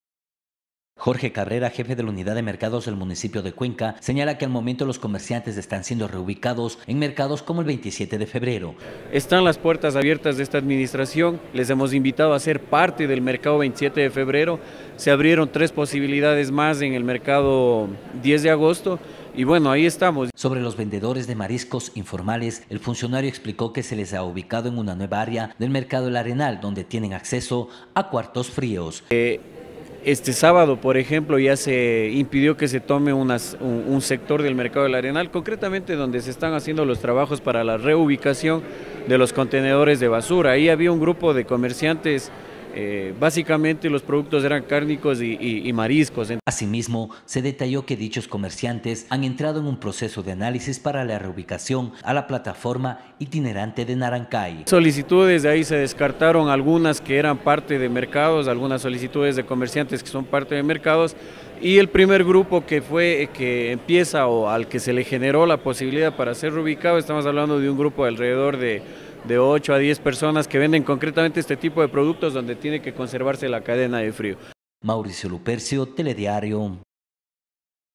COMERCIANTES INFORMALES SON REUBICADOS.
COMERCIANTES INFORMALES DEL MERCADO MAYORISTA EL ARENAL ESTAN SIENDO REUBICADOS EN DIFERENTES CENTROS DE ABASTO DE LA URBE HASTA QUE FINALICE LA CONSTRUCCIÓN DE LA PLATAFORMA ITINERANTE DE NARANCAY.